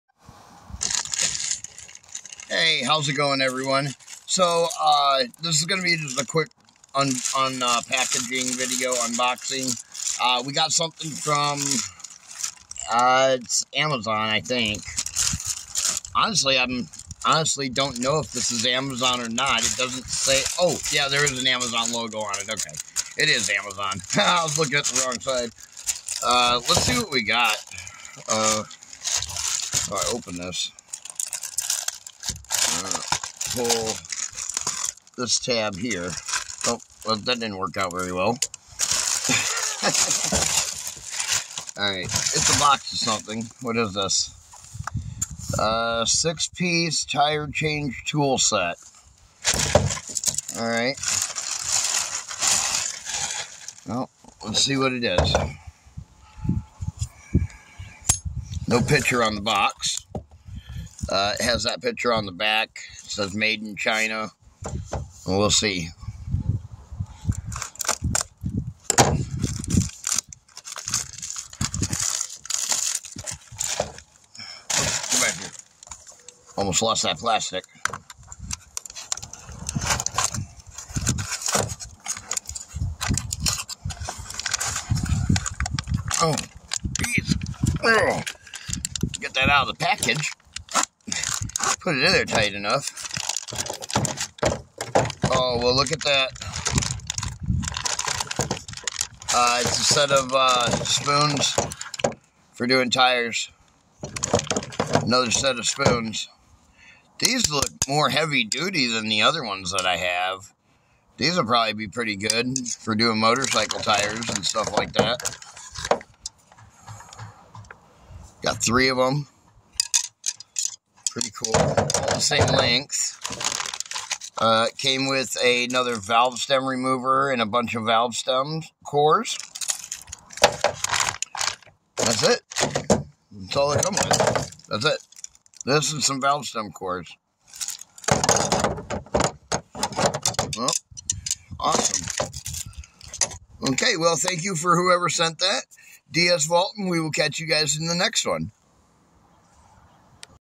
Unboxing Video #3
Got a package from Amazon someone sent us something off of our Amazon Wish List which you can find on our Facebook page big shout out and thank you for your generous donation we will put it to good use.